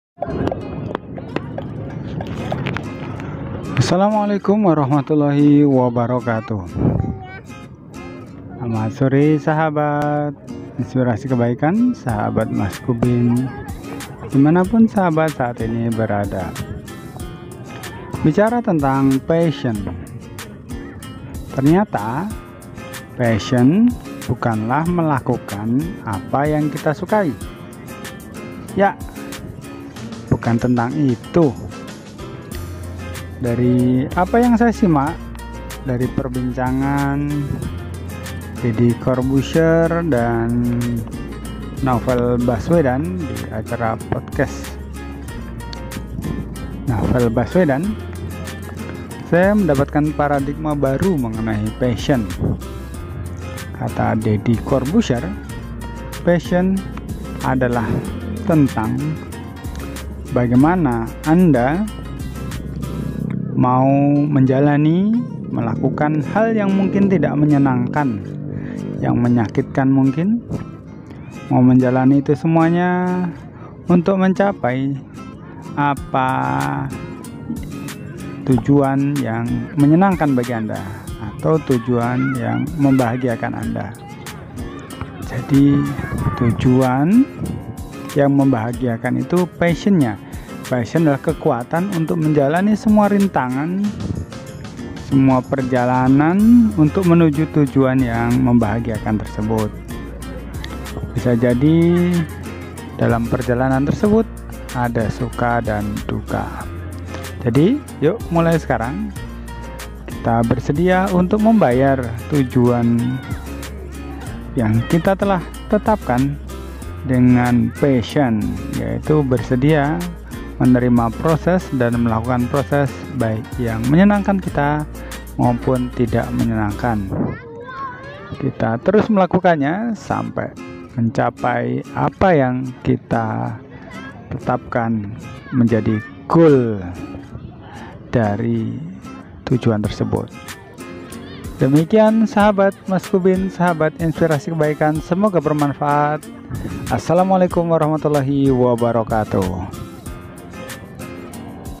Paradigma Bekerja dengan Passion yang Tepat menurut Deddy Corbuzier - apa itu passion
Passion itu ternyata bukan ketika Anda senang ketika melakukan sesuatu tetapi anda rela melakukan sesuatu walaupun hal tersebut tidak menyenangkan anda untuk sebuah misi tujuan yang akan membuat anda bahagia ketika tujuan tersebut tercapai

video motivasi ini akan mengubah persepsi anda mengenai pasiion...!!!
 hobi jadi duit endgame gita wirjawan kuliah atau kerja tokoh nasional